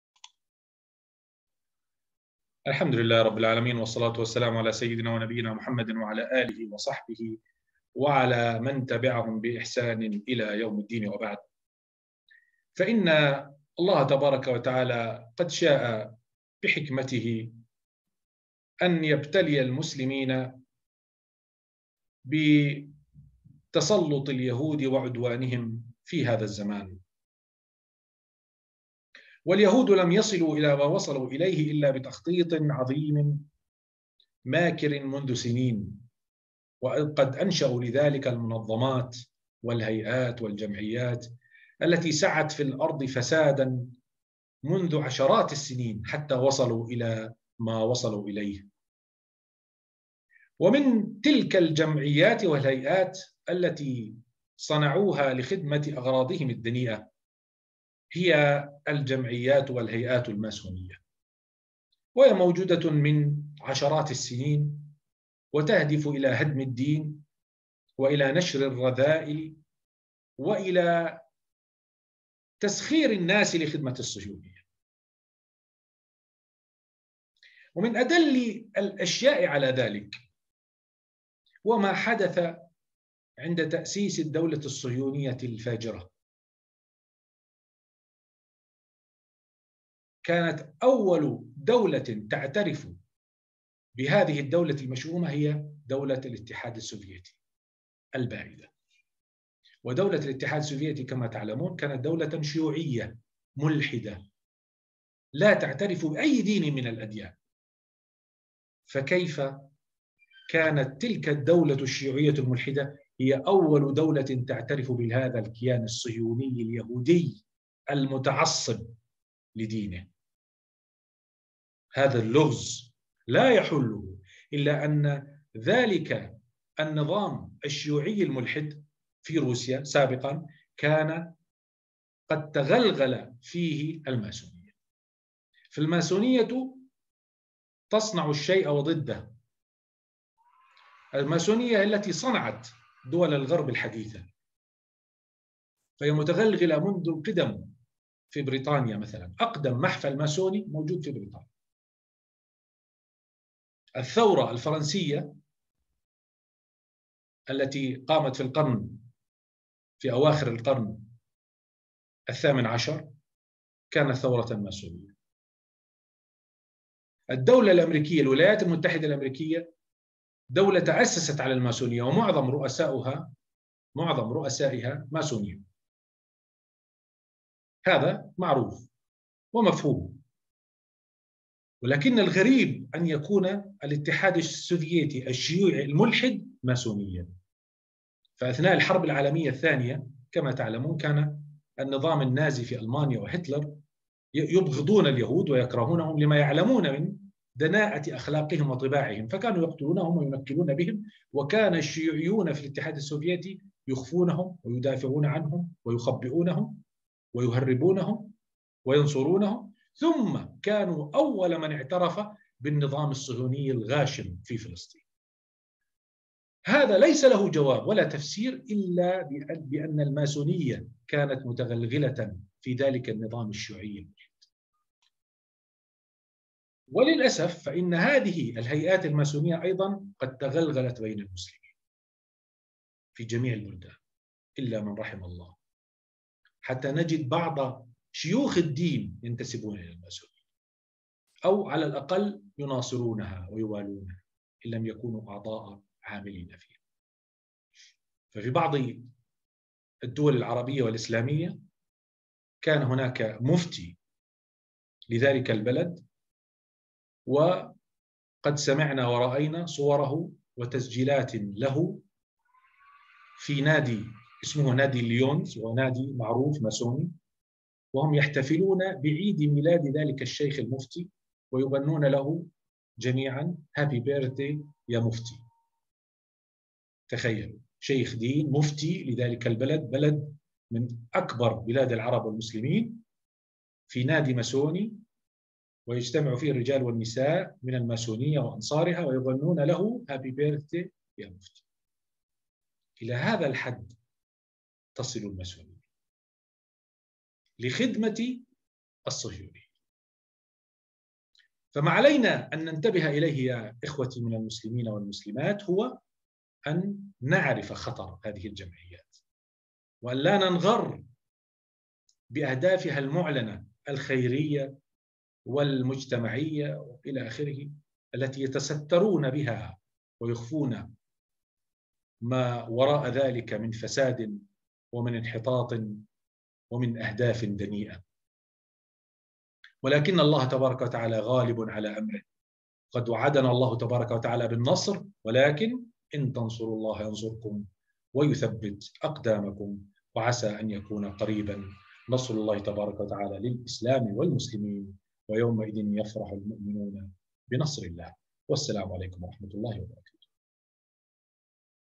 اخطبوط الماسونية
الماسونية المتغلغلة في جميع الدول لدعم الصهيونية